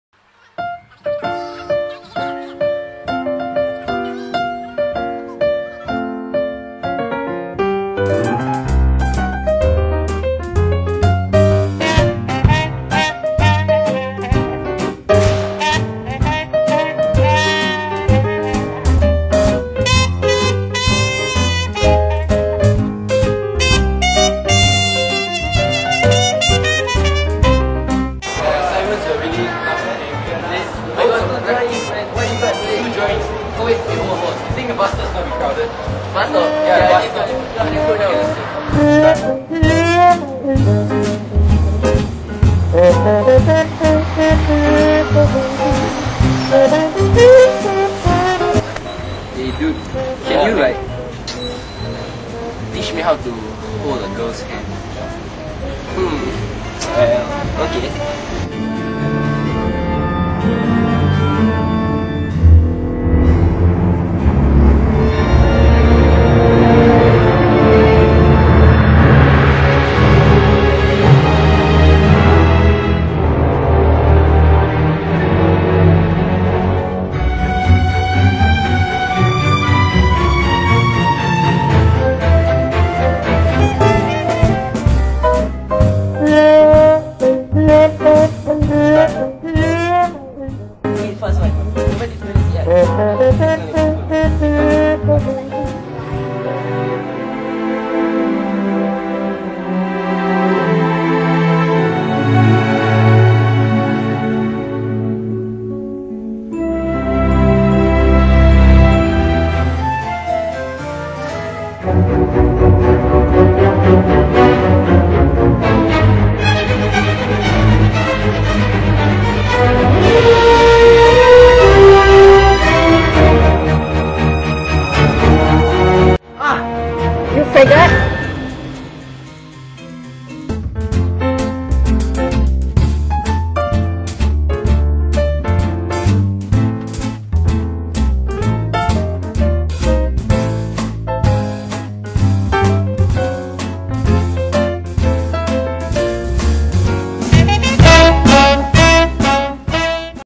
I'm not GAY
Thankz to the Production team and Talents

short video for video class about a guy,john, that loves a girl,jane, when he asked advice from his best buddy,kelvin, he was mistaken as a gay!

copyrighted material
verm & production team